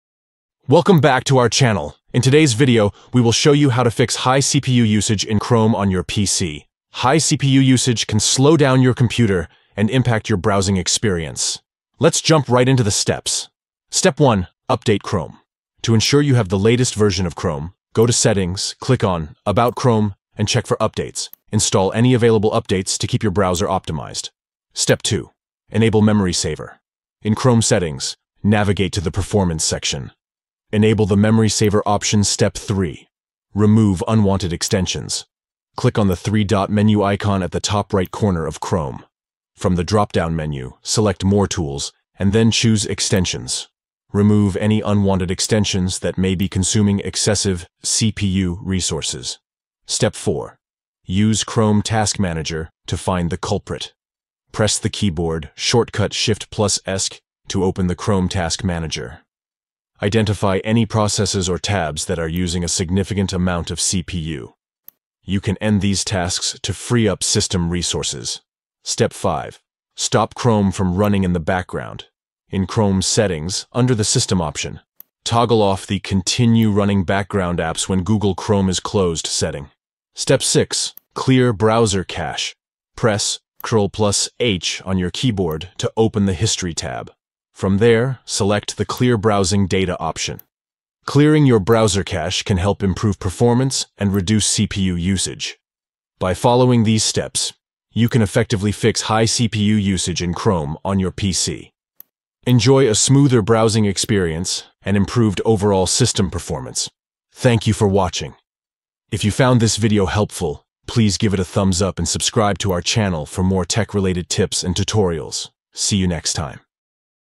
How to Fix Chrome High CPU Usage on PC
Experiencing high CPU usage in Chrome on your PC? Don't worry! In this tutorial, we'll show you how to fix it with simple steps.

Step 1: Update Chrome - Keep your browser optimized by going to settings, clicking on About Chrome, and checking for updates.

Step 2: Enable Memory Saver - Reduce strain on your CPU by going to Chrome Settings - Performance and enabling the Memory Saver option.

Step 3: Remove Unwanted Extensions - Click the three-dot menu icon, select More Tools, and choose Extensions. Remove any unnecessary extensions consuming CPU resources.

Step 4: Use Chrome Task Manager - Identify the culprit by using the keyboard shortcut Shift + Esc to open the Chrome Task Manager. End any processes or tabs using excessive CPU resources.

Step 5: Stop Chrome from running in the background - In Chrome Settings, under the System option, toggle off "Continue running background apps when Google Chrome is closed" to prevent unnecessary CPU usage.

Step 6: Clear browser cache - Improve performance and reduce CPU usage by pressing Ctrl + H, selecting Clear browsing data, and clearing your browser cache.

Follow these step-by-step instructions to fix high CPU usage in Chrome on your PC. Enjoy a smoother browsing experience and improved system performance.